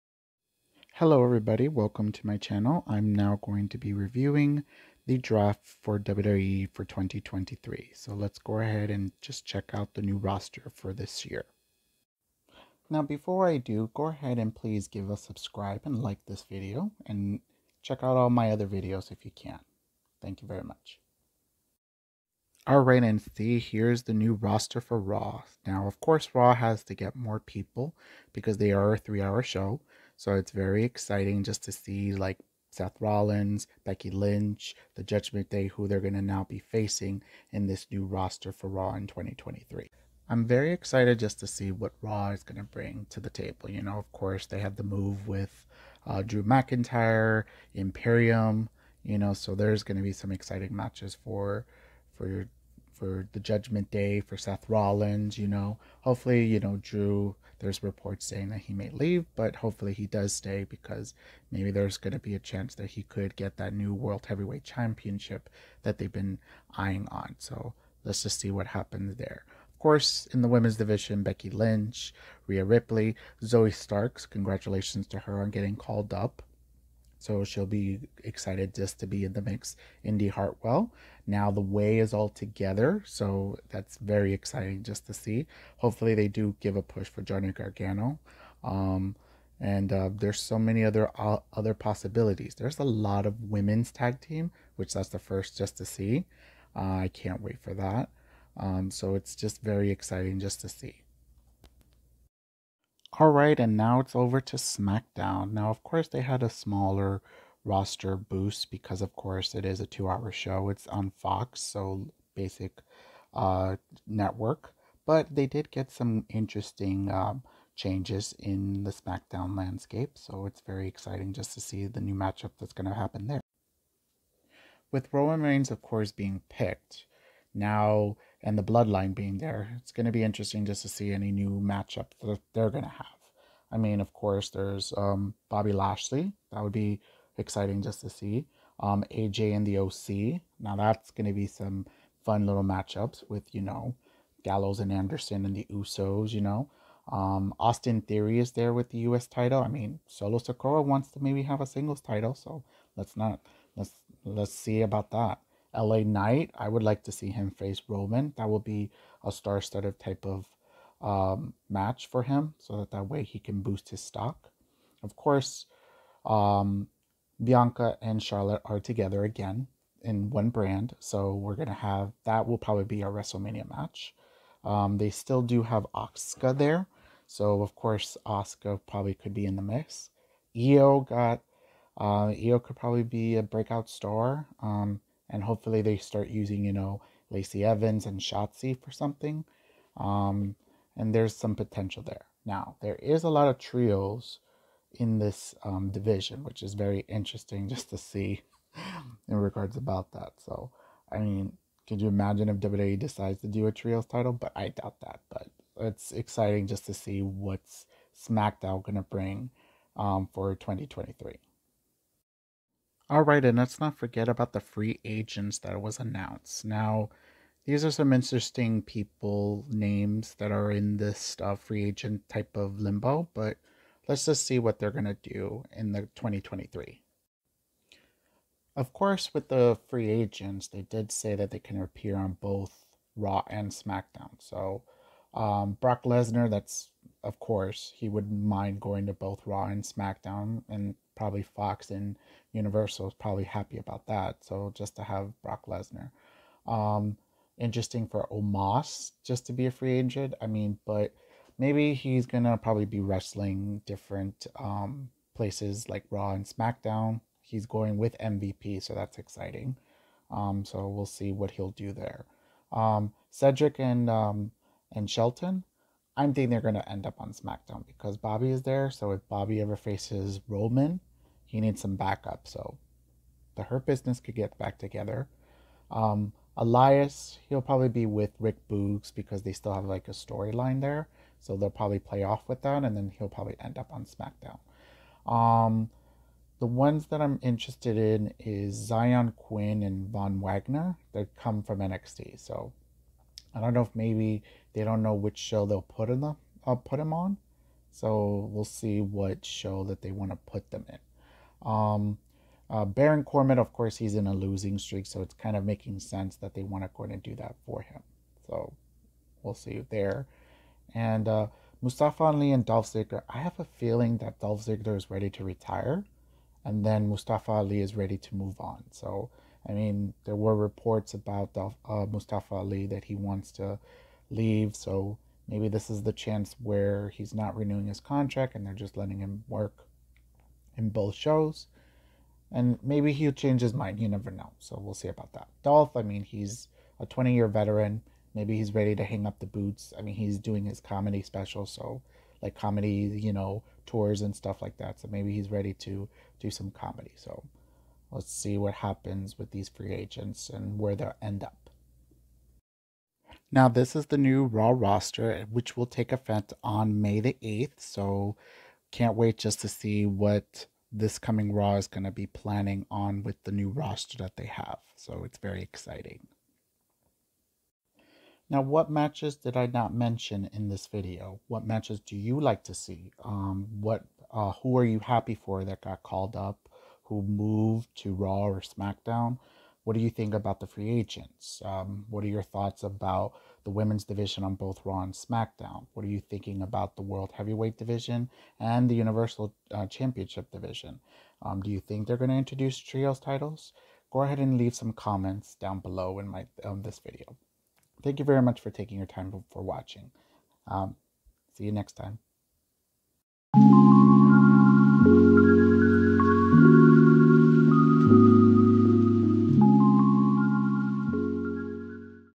WWE Draft 2023 Review
My thoughts on the new rosters for WWE RAW and Smackdown.